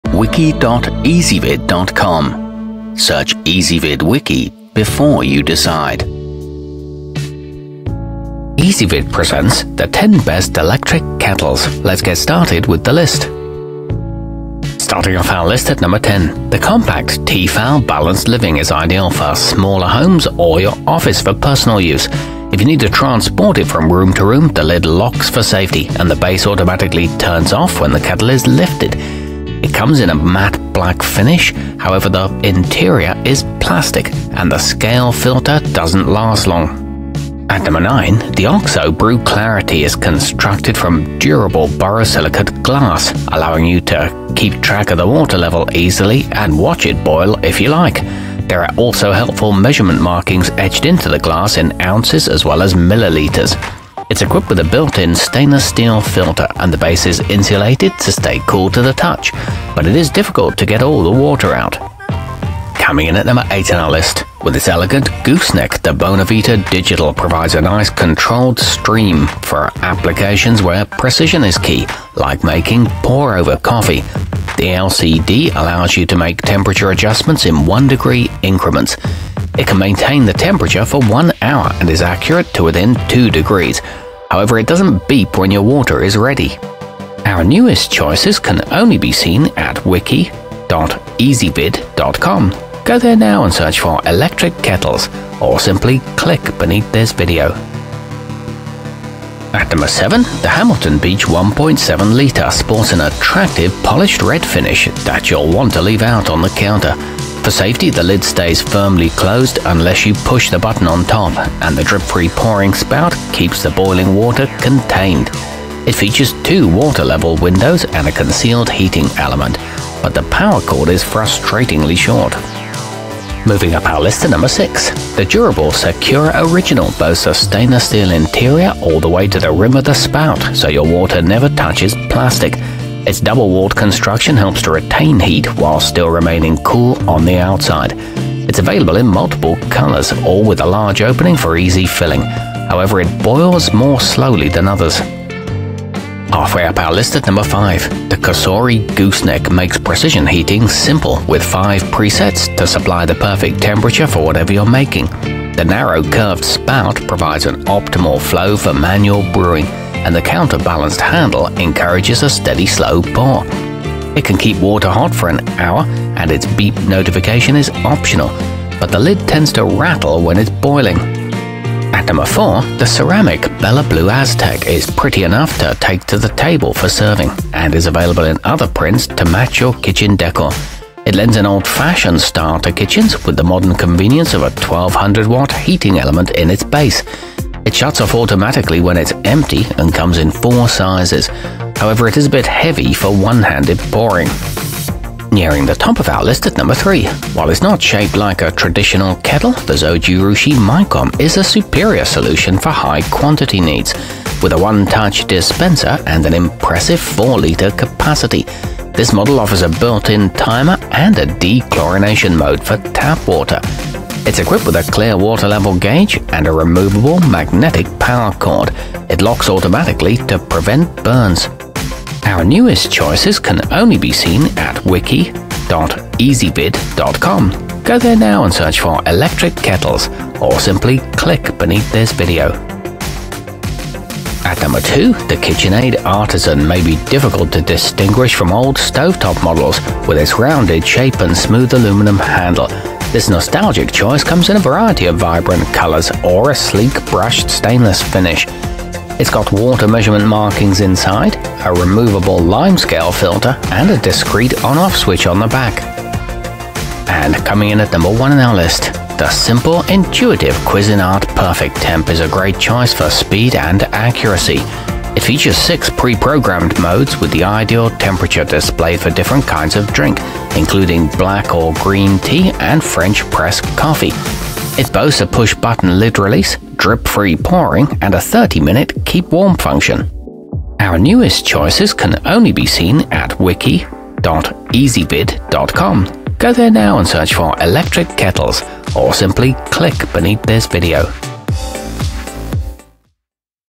10 Best Electric Kettles 2020
Our complete review, including our selection for the year's best electric kettle, is exclusively available on Ezvid Wiki.

Electric kettles included in this wiki include the t-fal balanced living, oxo brew clarity, cuisinart perfectemp, kitchen aid artisan, bonavita digital, secura original, bella blue aztec, hamilton beach 1.7 liter, zojirushi micom, and cosori gooseneck.